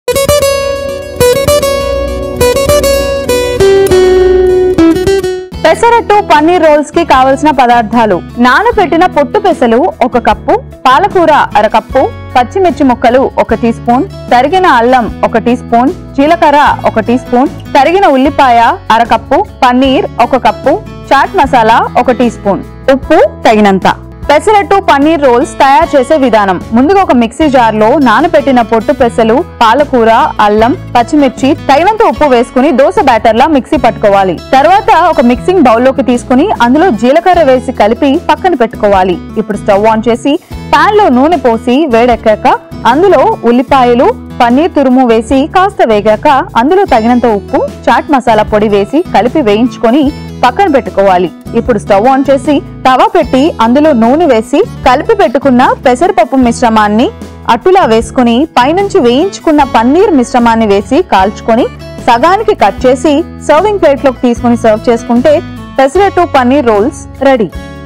Pesarattu Paneer Rolls | Quick Recipes | ETV Life
Pesarattu Paneer Rolls is a flavorful dish prepared using soaked green gram, spinach, green chilies, onions, paneer, and other simple ingredients.